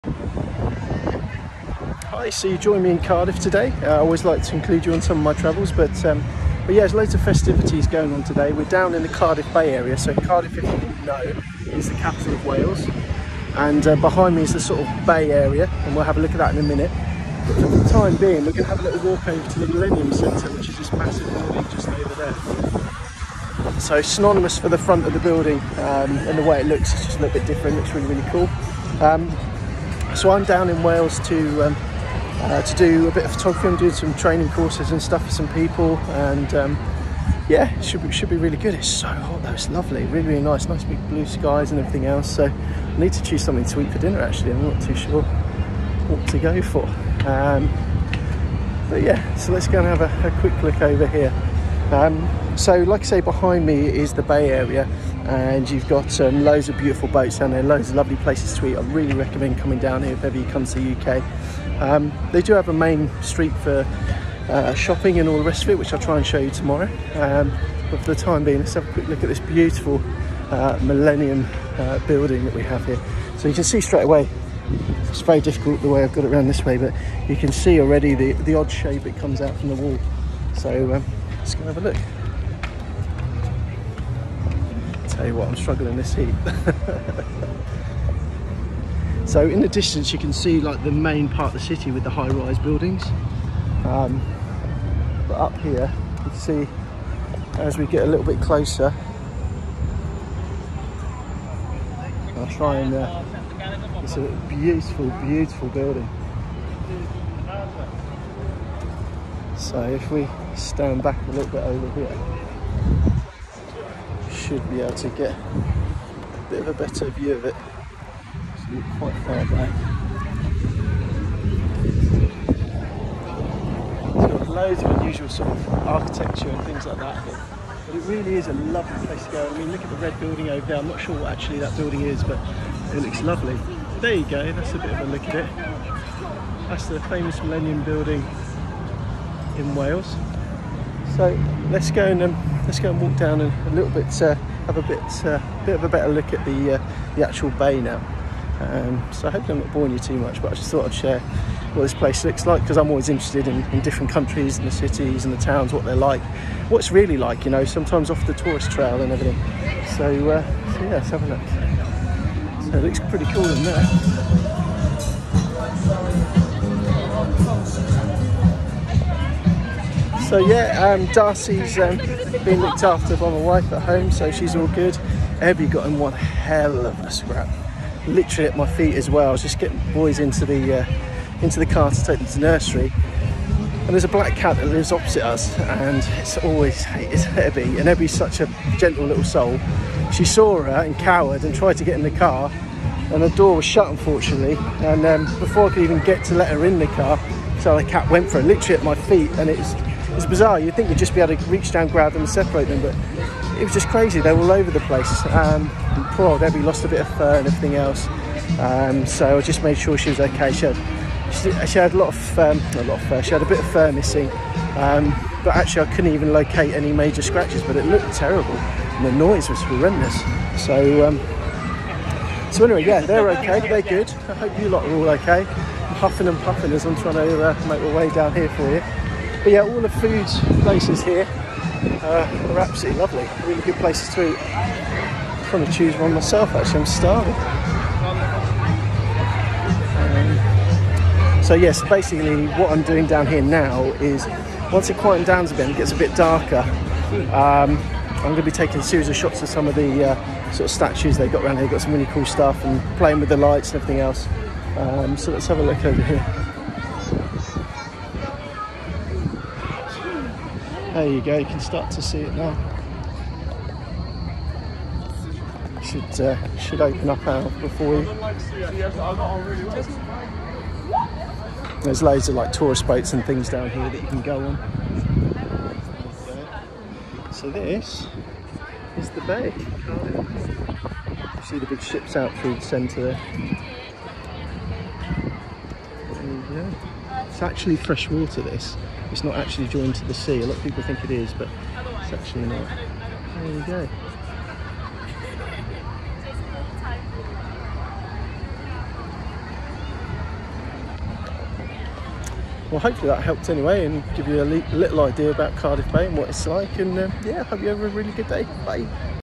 23 July 2021
A quick look around Cardiff Bay